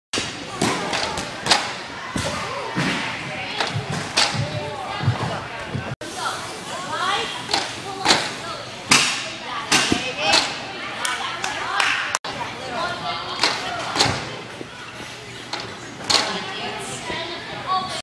Maggie McAvoy 2014 - New Side Aerial Combination
Maggie McAvoy - Level 10 - Southeastern Gymnastics
Cat leap side aerial to sisone jump. New beam combination
Junior - Class of 2014